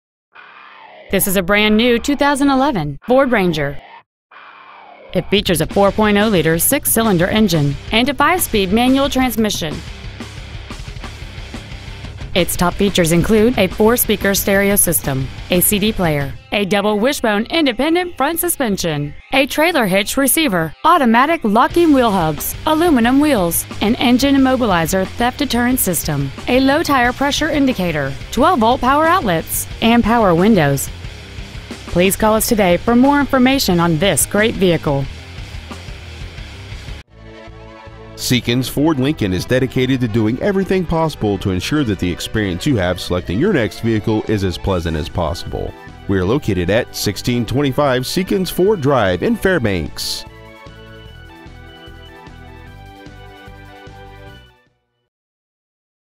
2011 FORD RANGER Fairbanks AK
We have been honored to serve the Fairbanks AK area , we promise that your experience at our dealership will exceed your expectations !

 Year : 2011
 Make : FORD
 Model : RANGER
 Engine : 4.0L V6
 Trans . : MANUAL 5SPD
 Exterior : BLACK
 Miles : 5
 Interior : MEDIUM DARK FLINT
 Stock : 23705